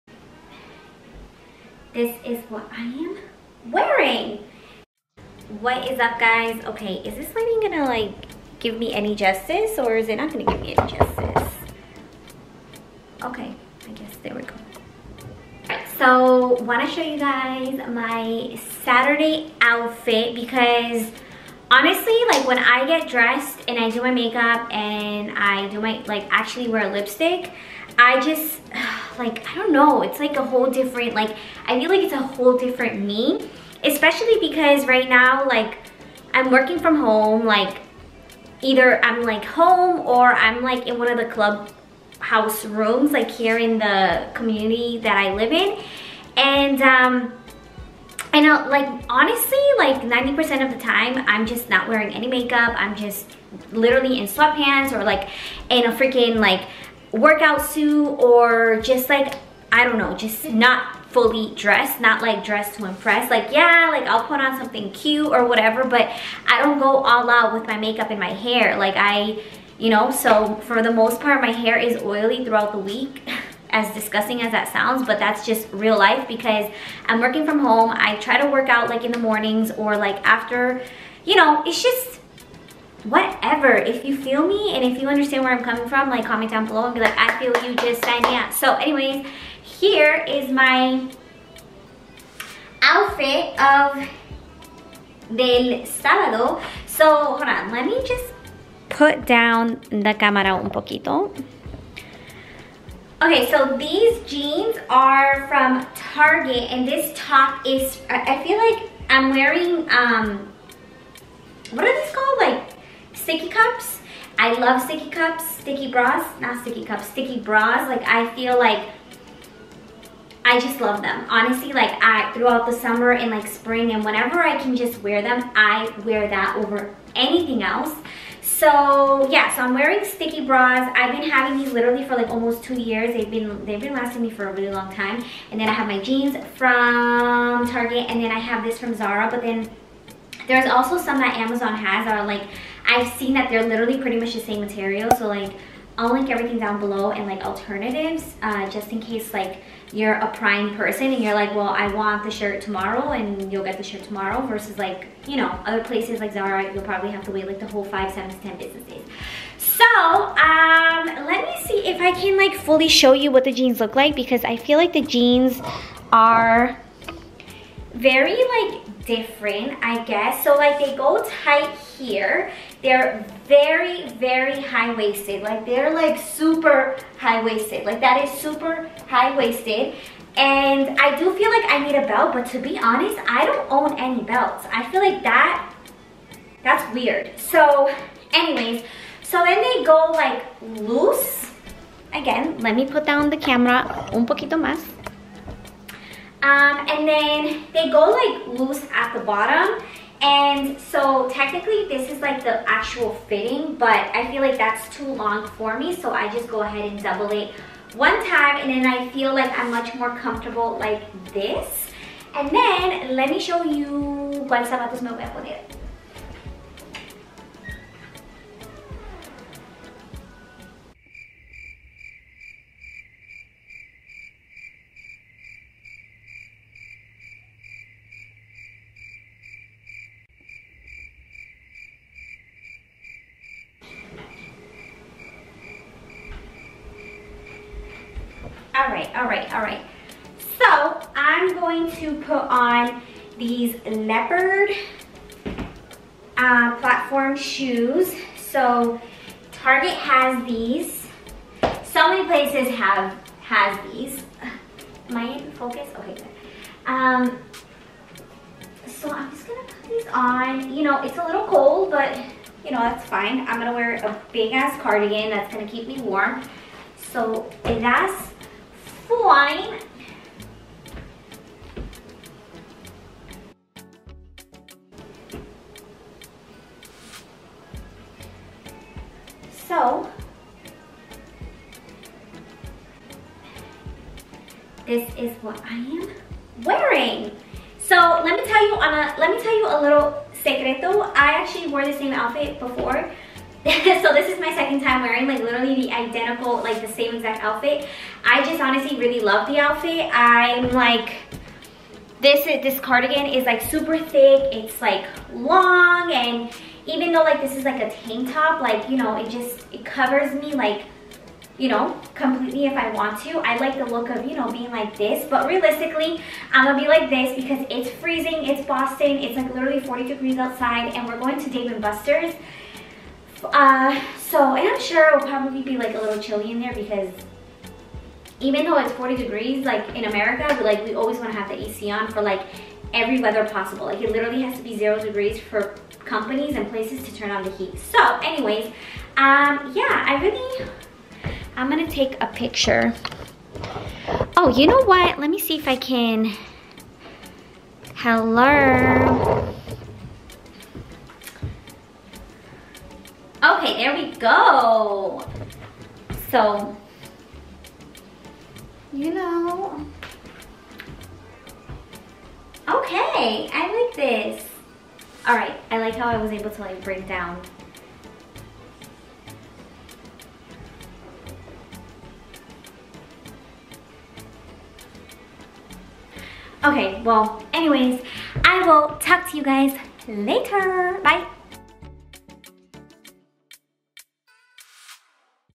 Get dressed with me after work from home week | Must have neutral pieces
Hellllerrrrr friends!!! So happy you are here and joining me to this weekend video! All details mentioned in todays fashion inspo-ish video is below! Xoxoxoxo Jessenia